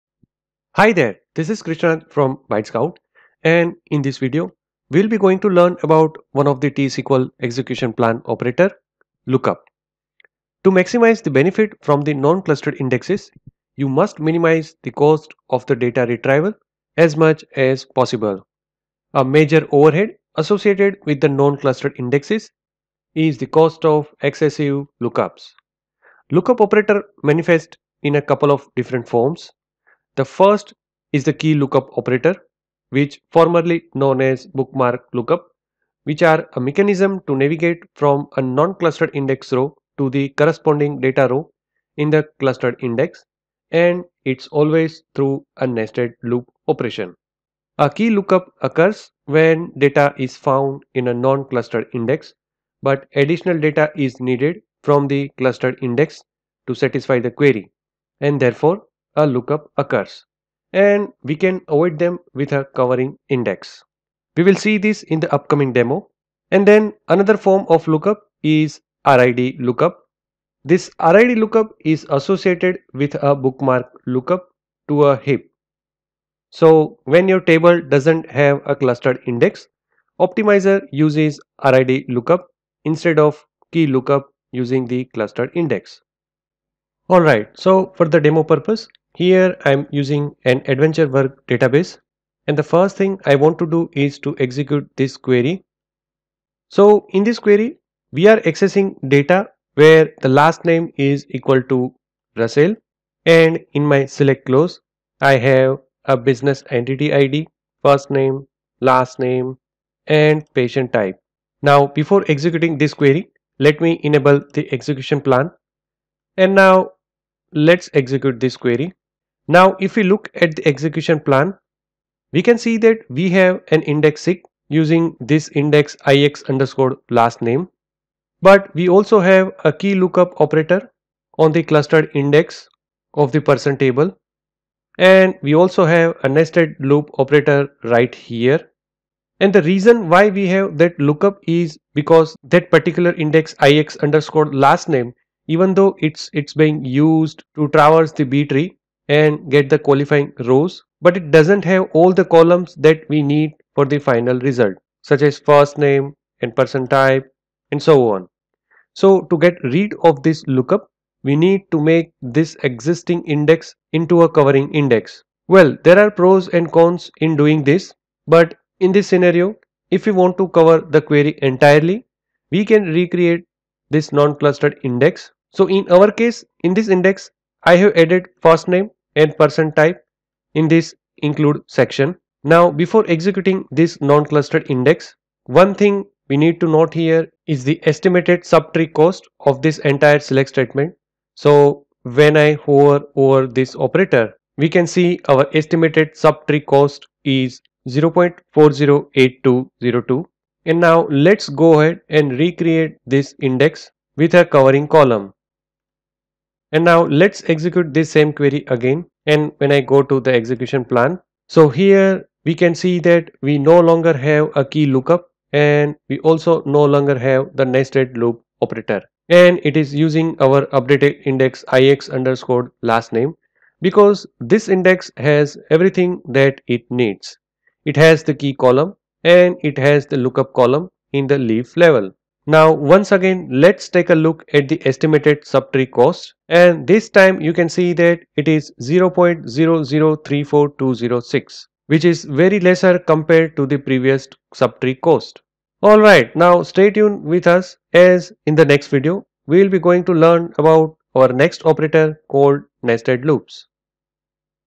TSQL Execution Plan Operator - Lookup
In SQL Server Lookup operator manifests in a couple of different forms. The first is the key lookup operator which formerly known as bookmark lookups which are a mechanism to navigate from a nonclustered index row to the corresponding data row in the clustered index and it's always through a nested loop operation.

A key lookup occurs when data is found in a non-clustered index, but additional data is needed from the clustered index to satisfy the query and therefore a lookup occurs and another form of lookup is RID lookup. This RID lookup is associated with a bookmark lookup to a heap. In this video, we'll be going to learn about these two operators.